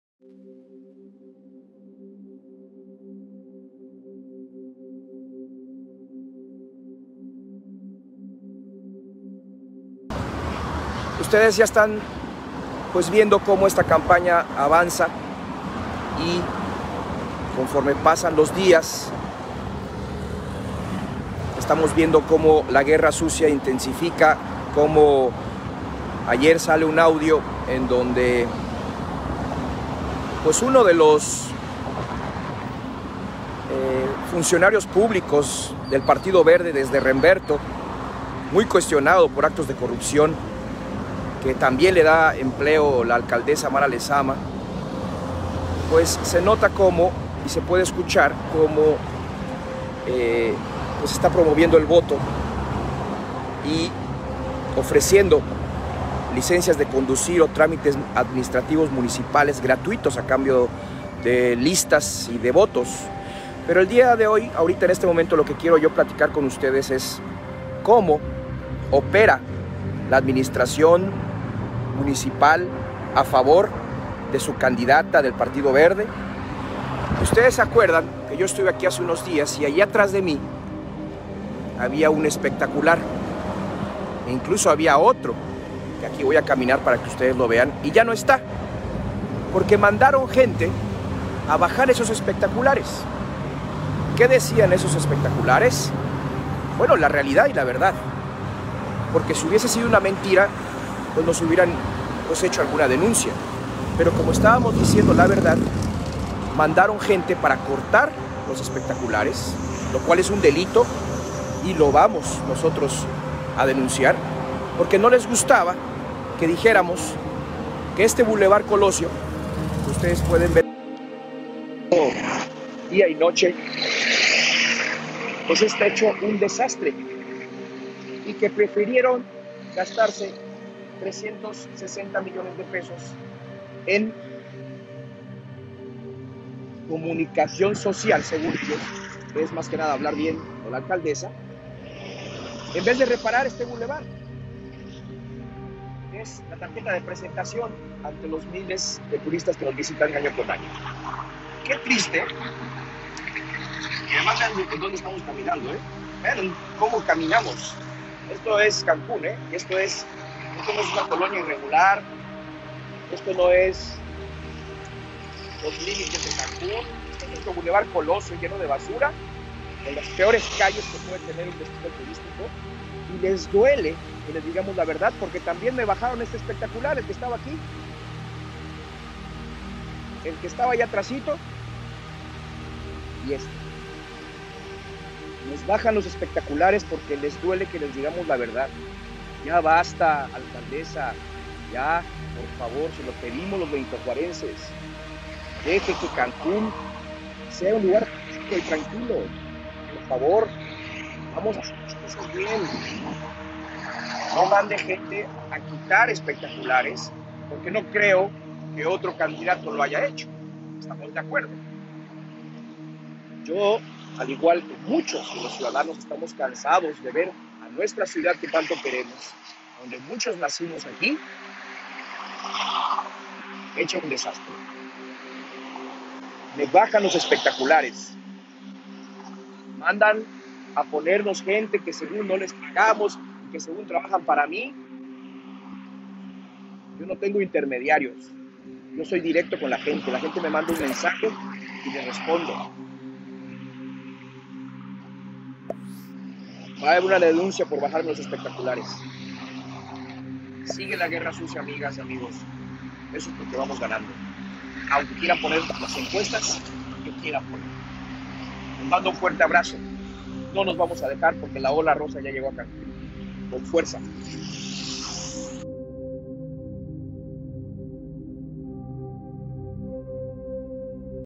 DESTRUYEN ESPECTACULARES DE ISSAC JÁNIX; “GUERRA SUCIA” EN REDES
Rodrigo Alcázar Urrutia, quien fue muy cuestionado en su actuar con actos de corrupción y que hoy está en el Ayuntamiento con Mara Lezama, promueve el voto entre taxistas a favor del verde, a quienes ofrece descuentos y muchos beneficios.